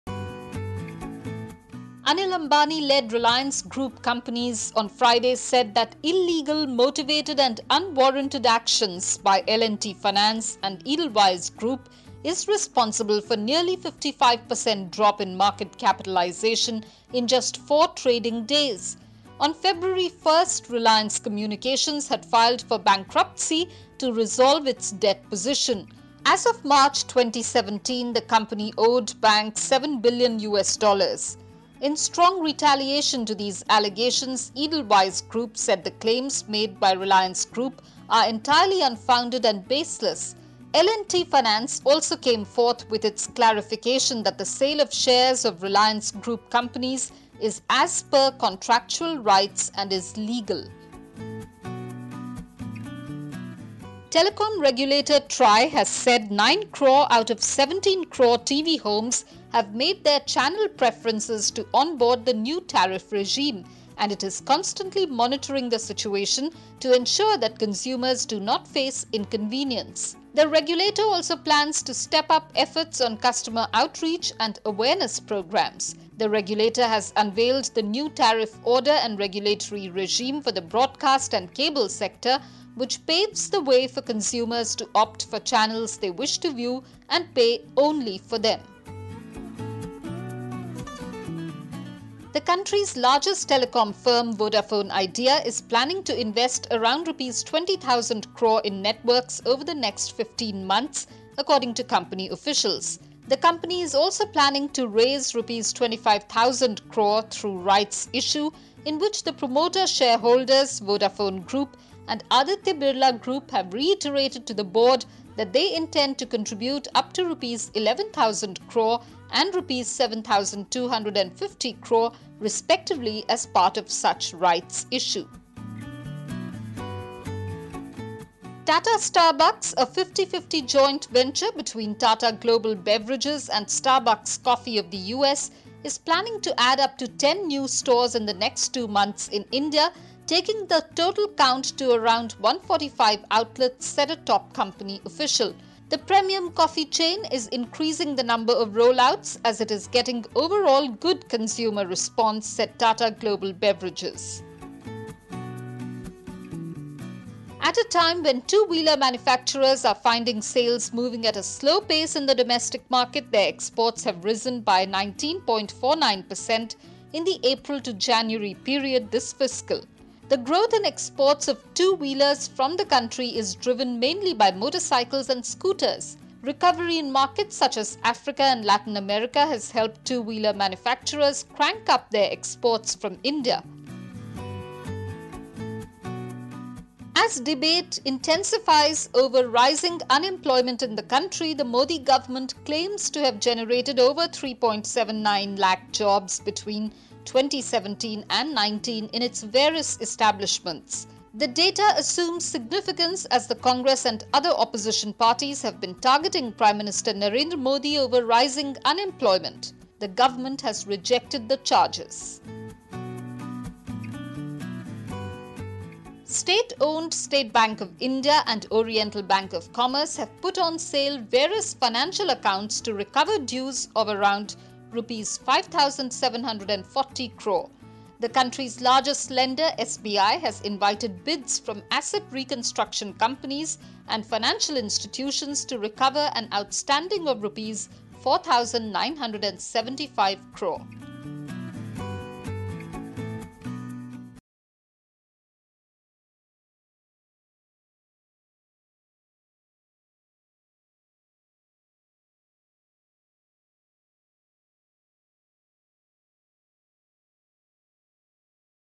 Anil Ambani's Reliance Group loses Rs 13,000 cr, Starbucks to open 10 new stores
Anil Ambani-led Reliance Group companies said that illegal, motivated and unwarranted actions by L&T Finance and Edelweiss group is responsible for nearly 55 per cent drop in market capitalisation, two-wheeler exports from India rise 19.5% and other news on News Blast